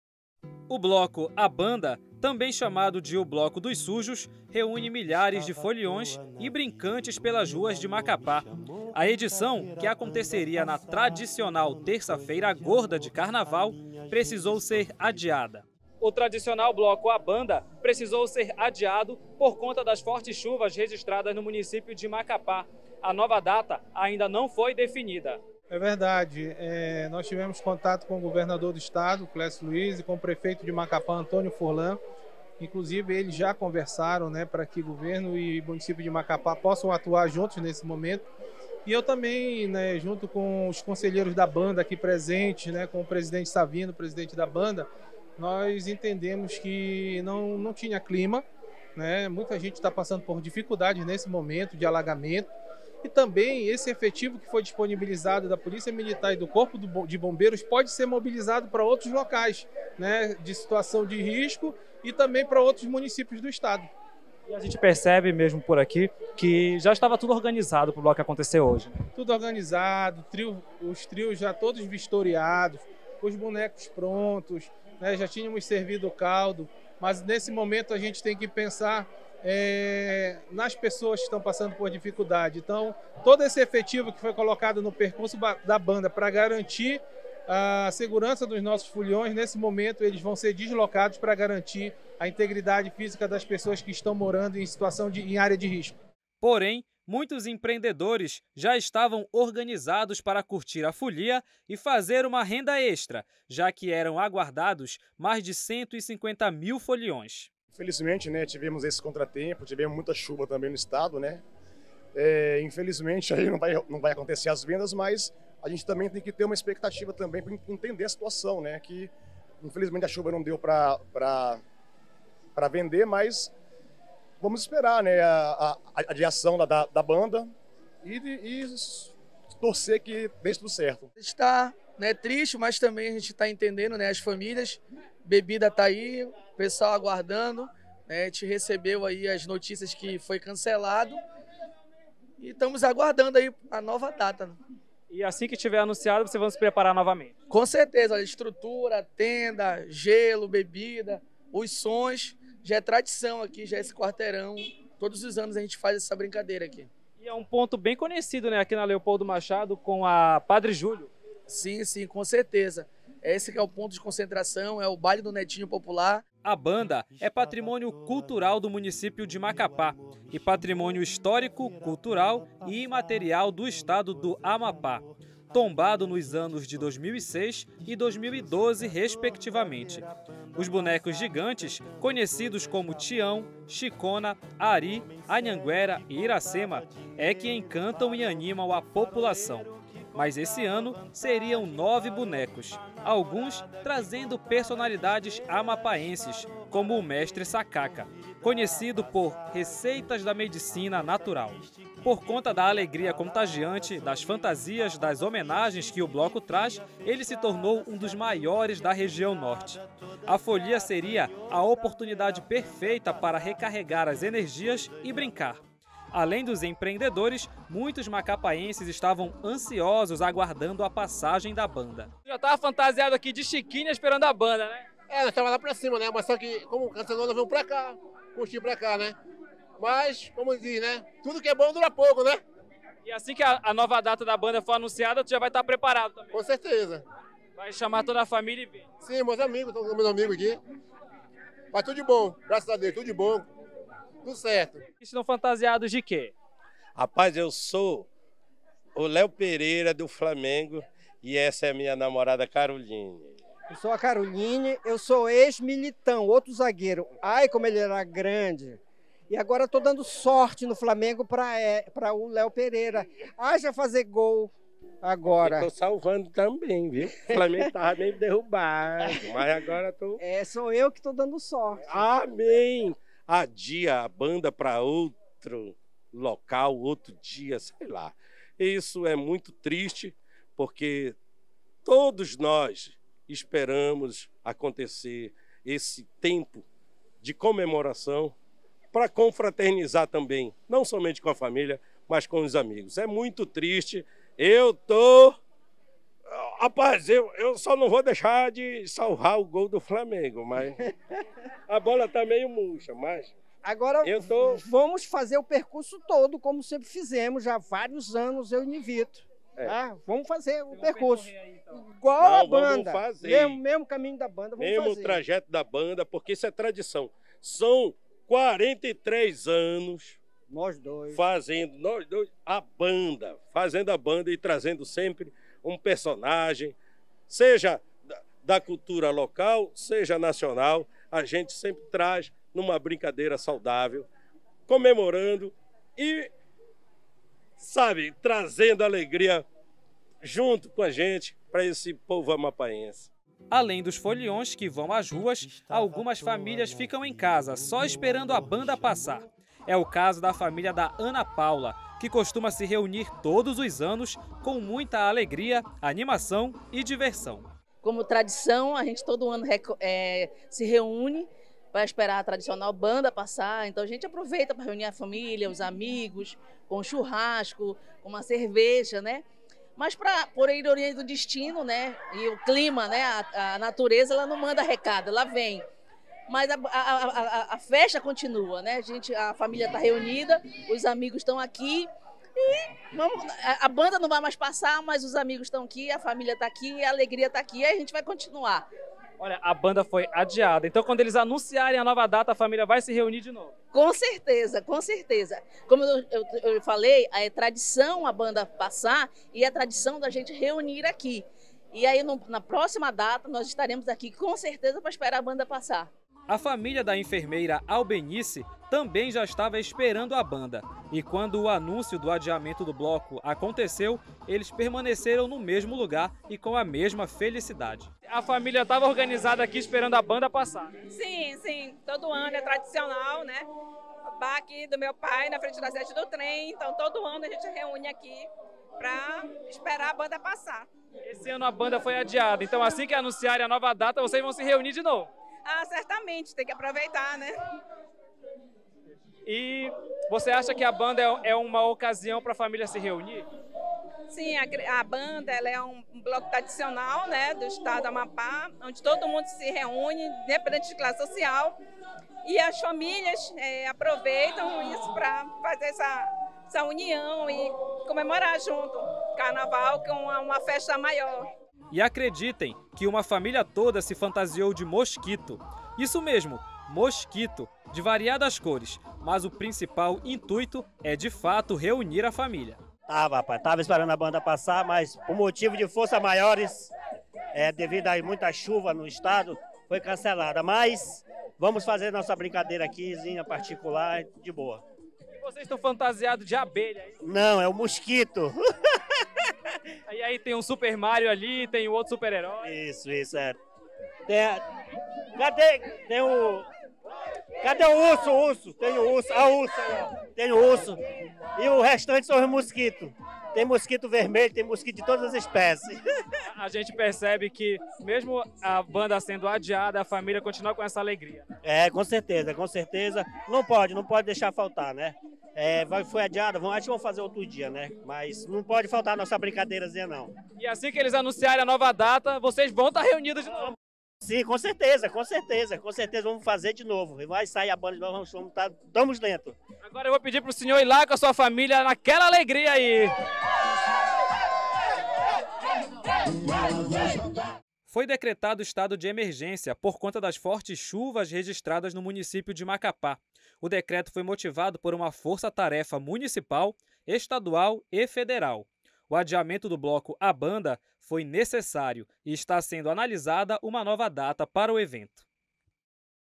"A Banda" é adiada por conta das fortes chuvas que aconteceram em Macapá, na terça-feira, 13
O bloco "A Banda", também chamado de "Bloco dos Sujos", reúne milhares de foliões e brincantes pelas ruas de Macapá. A edição que aconteceria na tradicional terça-feira "gorda" de carnaval, precisou ser adiada